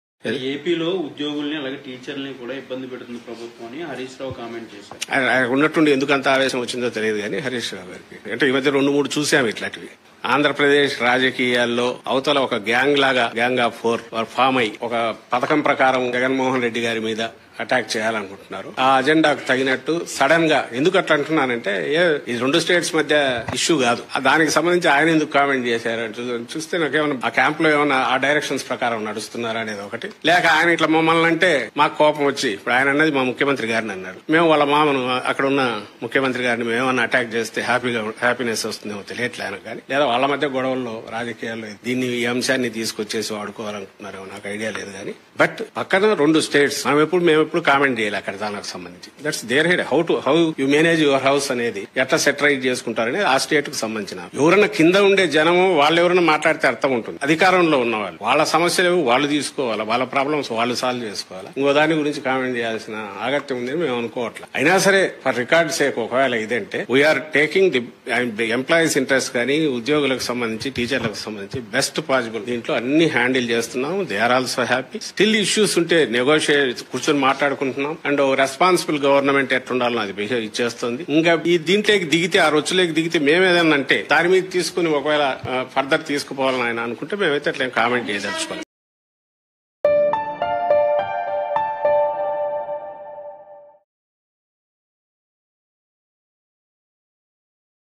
YCP vs TRS | మంత్రి హరీశ్ రావ్ కామెంట్స్... సజ్జల కౌంటర్
తెలంగాణ మంత్రి హరీష్ రావ్ వ్యాఖ్యలకు ఏపీ ప్రభుత్వ సలహాదారు సజ్జల రామకృష్ణారెడ్డి కౌంటర్ ఇచ్చారు. గురువారం ఓ సందర్భంలో తెలంగాణ మంత్రి హరీశ్ రావు .. ఏపీలో టీచర్లు, వ్యవసాయ మోటార్లకు మీటర్ల గురించి స్పందించారు. ఇప్పుడు ఆ కామెంట్సే ఇరు రాష్ట్రాల మధ్య డైలాగ్ వార్‌కు కారణమయ్యాయి. హరీష్ రావు ఎందుకు అలా మాట్లాడారో అర్ధం కావడం లేదన్న సజ్జల.. ఇది రెండు రాష్ట్రాల మధ్య అంశం కాదని స్పష్టం చేశారు. సీఎం జగన్‌ను విమర్శించి రెచ్చగొట్టాలని చూస్తున్నట్టుగా కన్పిస్తుందని అనుమానించారు. ఏదో గ్యాంగ్‌తో జతకట్టి హరీష్ రావు మాట్లాడినట్టుగా అనిపిస్తుందని వ్యాఖ్యానించారు. మరోవైపు మంత్రులు బొత్స, అమర్ నాథ్ కూడా